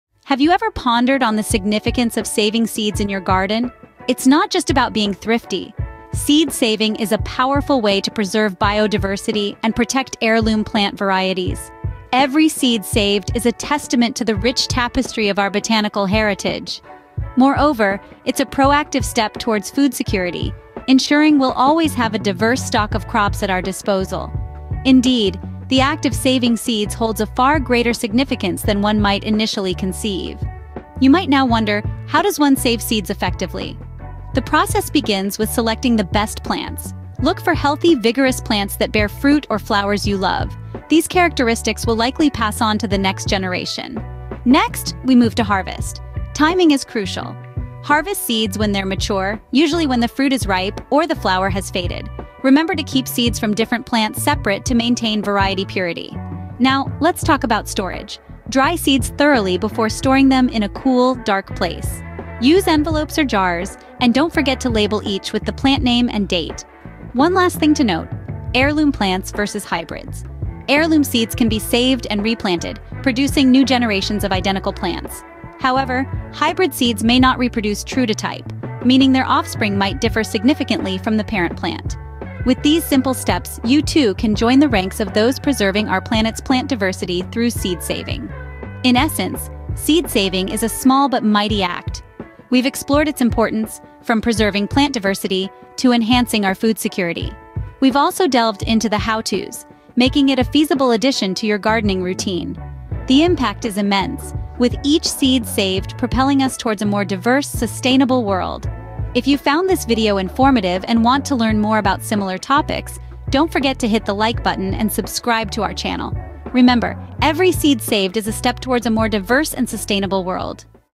Seed Saving: Preserving Plant Varieties
🌱 Dive into the fascinating world of seed saving and learn how to preserve the diversity of plant varieties in your garden! This informative video will walk you through the process of collecting, storing, and sharing seeds from your favorite plants, empowering you to become a steward of biodiversity.

Discover the importance of seed saving in maintaining resilient and sustainable food systems, as well as preserving heirloom and rare plant varieties for future generations. Explore different seed-saving techniques tailored to various types of plants, including vegetables, herbs, and flowers, and gain valuable tips for ensuring seed viability and germination success.

Learn how to identify and select plants for seed saving, harvest seeds at the right time, and properly clean and store them for long-term viability. Get insights into the best practices for labeling and organizing your seed collection, as well as how to share seeds with friends, family, and fellow gardeners to promote diversity and resilience in community gardens and beyond.

Whether you're a seasoned gardener looking to expand your seed-saving skills or a beginner eager to embark on a journey of plant preservation, this video has everything you need to get started and contribute to the vibrant world of seed diversity.